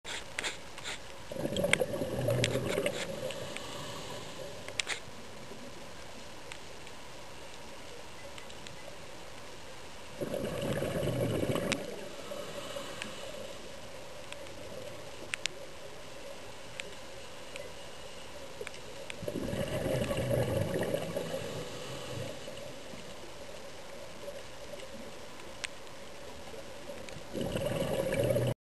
Spotted Eagle ray
I am swimming alongside a big spotted eagle ray at the Ice Cream dive site on the beautiful tropical island of Saipan.